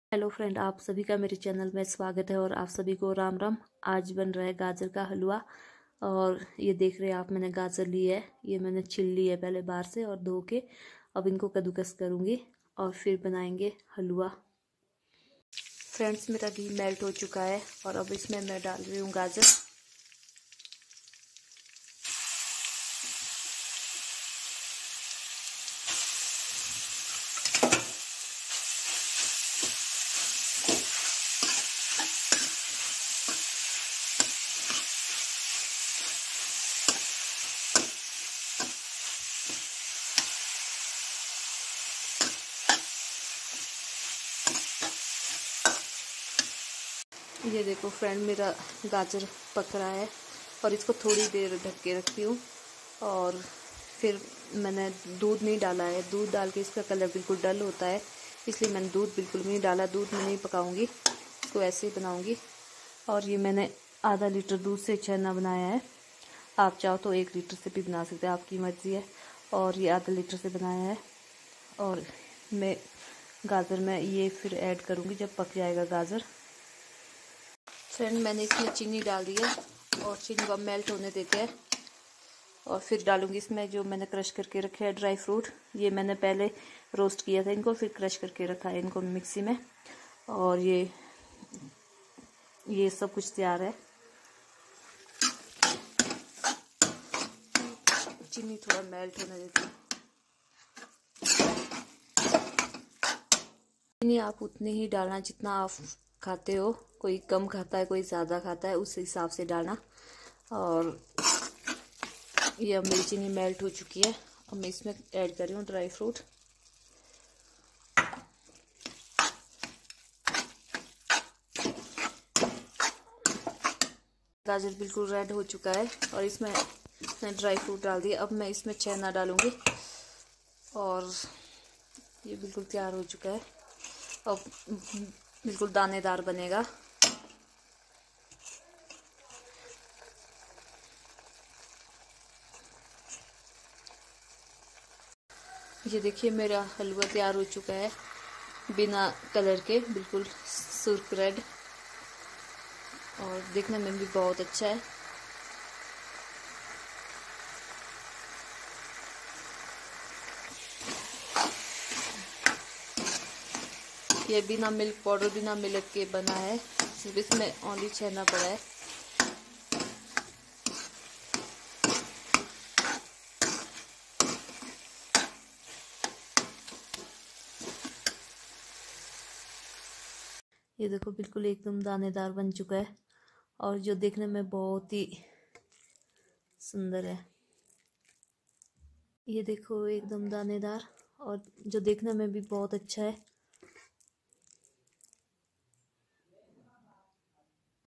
गाजर का हलवा बनाने की विधि 10 मिनट में बनाएं होटल जैसा बिना खोया बिना दूध के
gajar ka halwa recipe simple and delicious carrot halwa with out milk and color